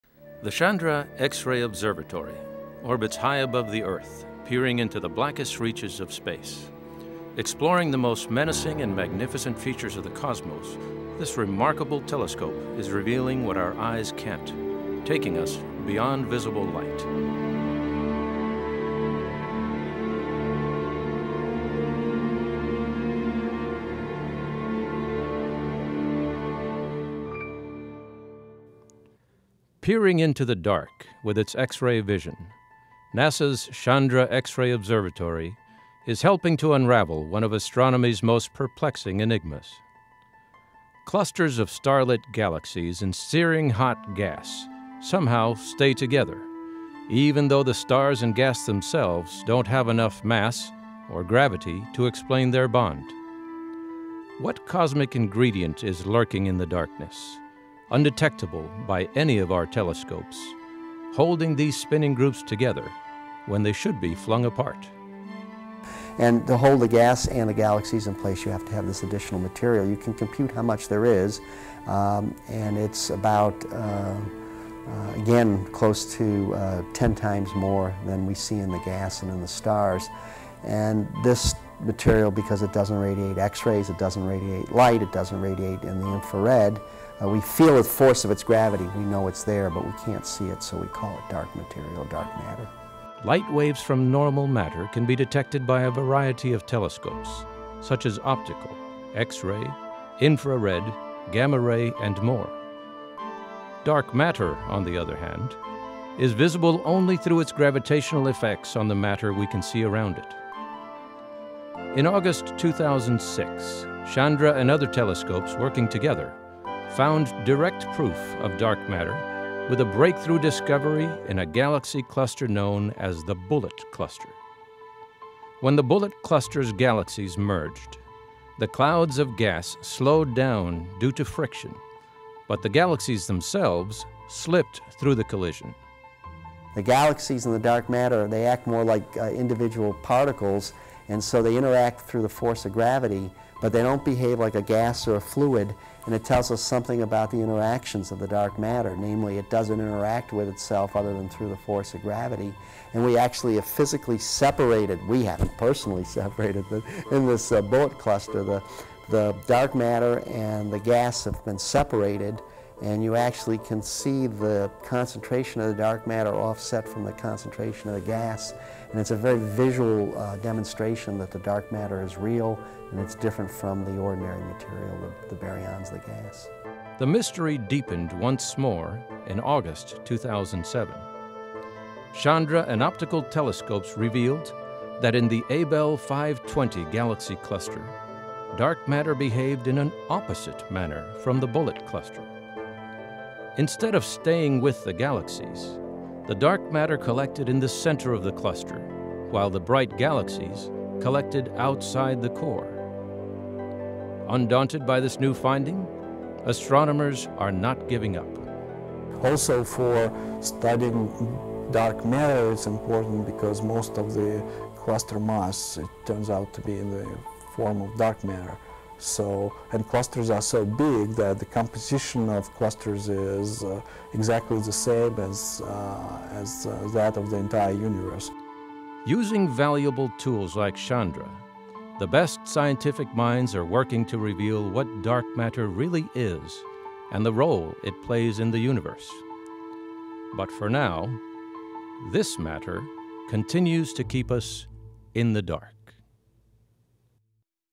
Chandra X-ray Observatory
Dark Matter
What cosmic ingredient is lurking in the darkness, undetectable by any of our telescopes, holding galaxies together when they should be flung apart?